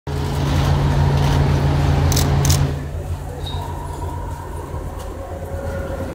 Roy Ngerng arriving at the State Courts
Roy Ngerng arriving at the State Courts for Amos Yee's trial. -- ST VIDEO: OLIVIA HO